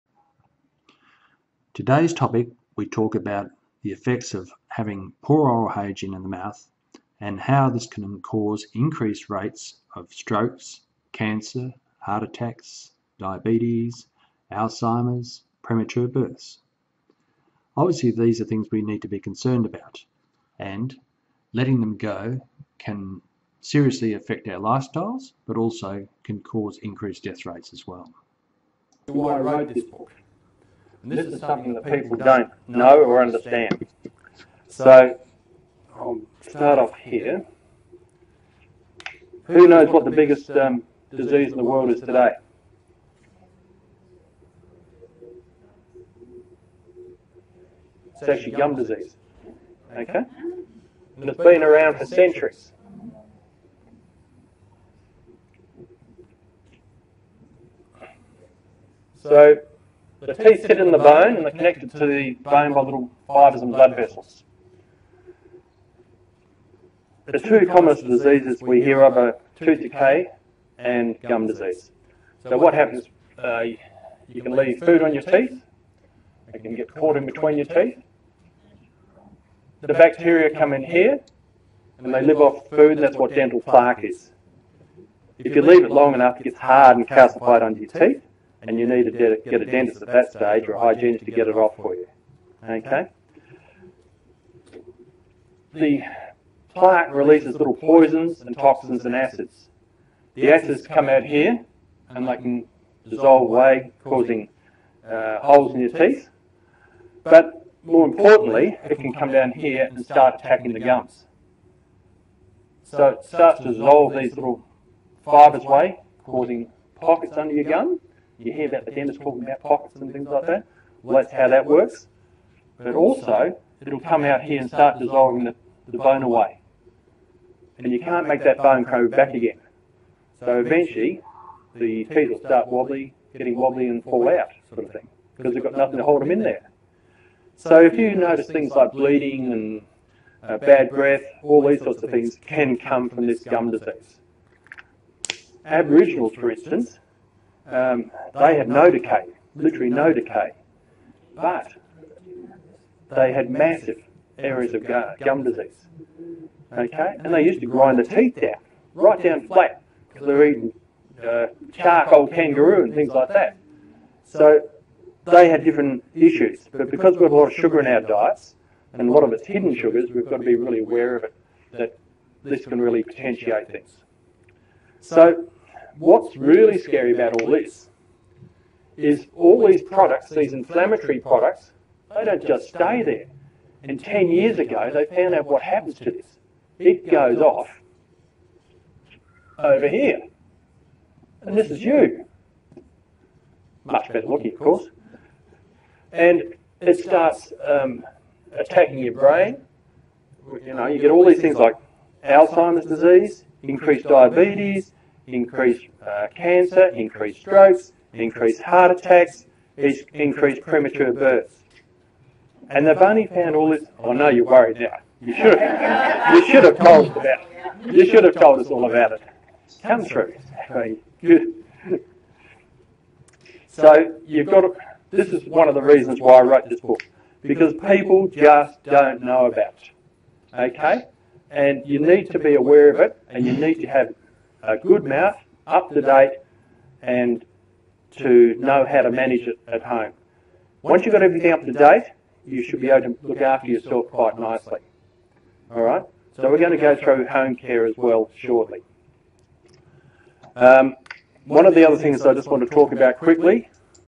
Poor oral health ramifications
Sorry but you need to know this as many people are having compromised health and dying from increased strokes, heart attack, cancer, diabetes, Alzheimers , premature birth etc